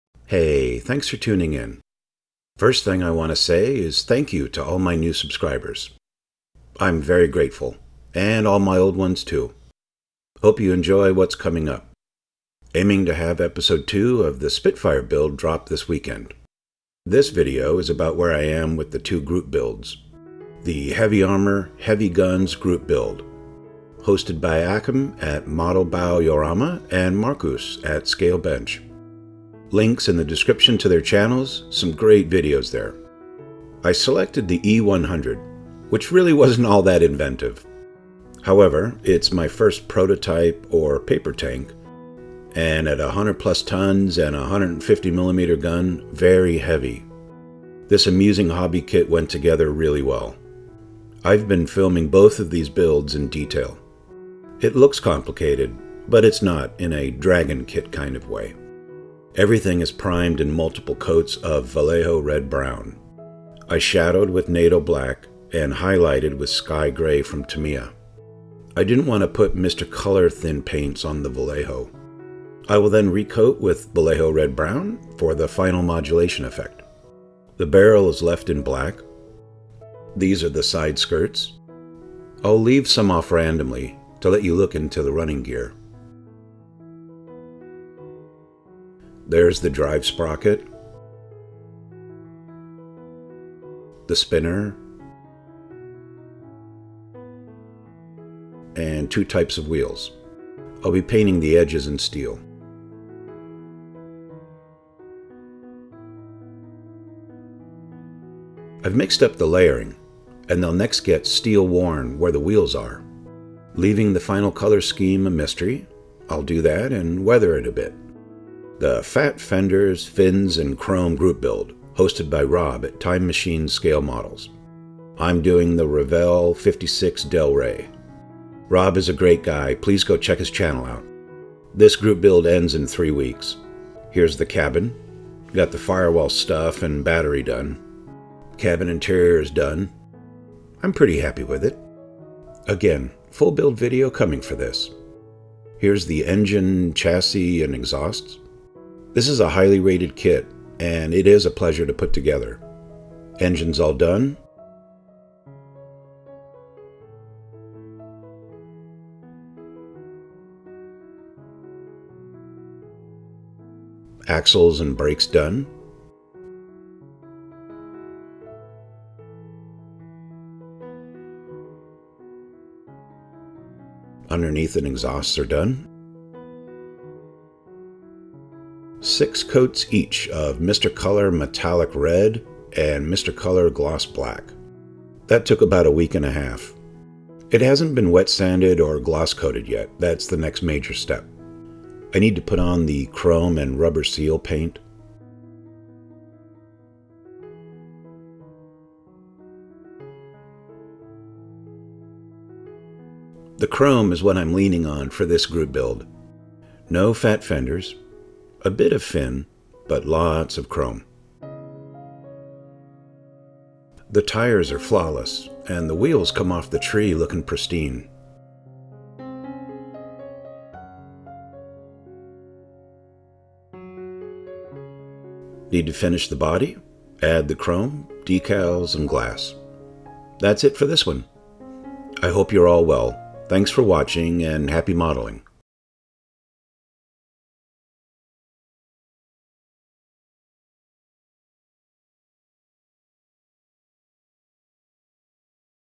Quick Group Build Update - Heavy Tank and Classic Car
Making progress on these two group builds. Also working on the Spitfire (Episode 02 this weekend) and some 60's muscle.

Hope everyone has a nice weekend. Be well and happy modeling.